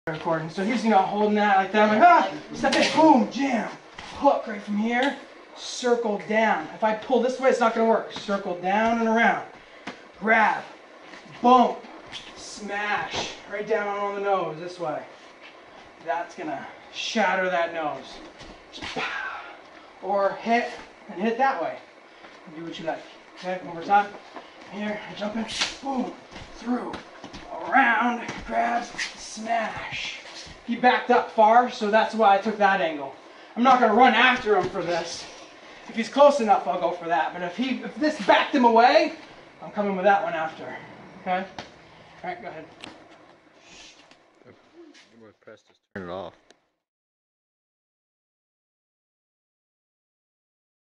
Defending of a Baseball Bat
Japanese Jujitsu Self-defense technique.￼ Defending off a baseball bat
For anyone that is confused or thinks that I am vulnerable by giving up my back here please check out my recent video explaining and demonstrating and explaining In further detail thanks and god bless! 🙏